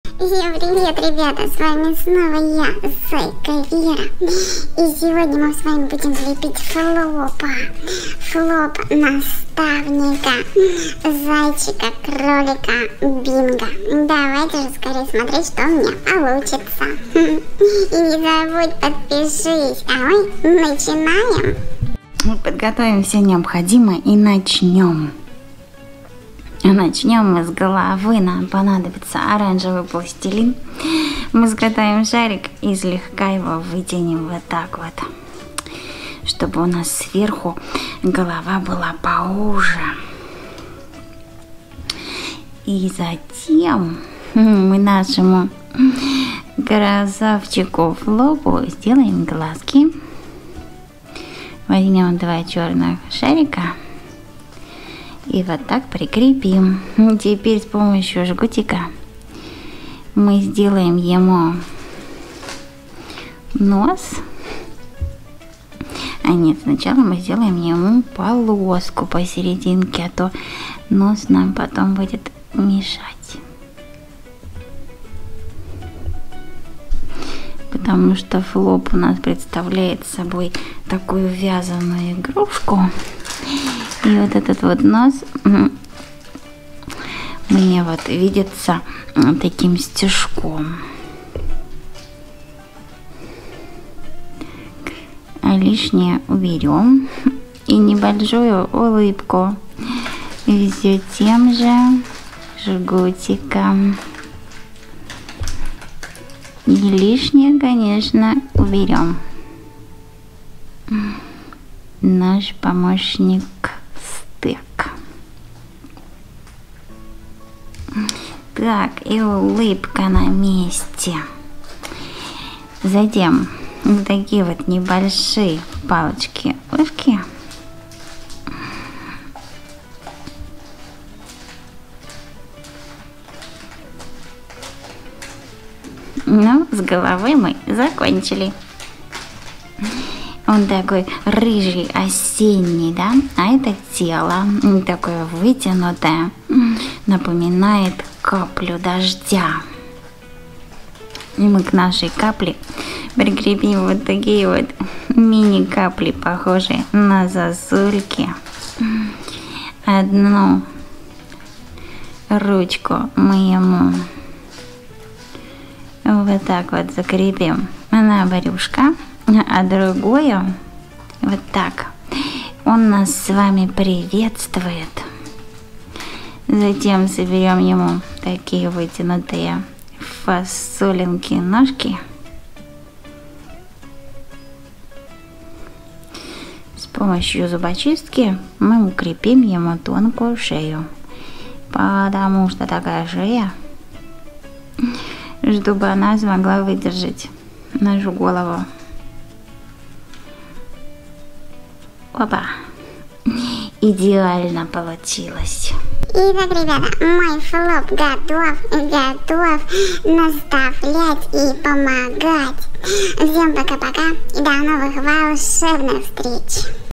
Лепим Флопа наставника Кролика Бинга из пластилина. Мультик Бинг\Bing bunny
Всем привет ребята!
Сегодня мы с вами рассмотрим как слепить Флопа наставника Кролика Бинга из мультфильма Бинг)
Легкие поделки для детей/Видео лепка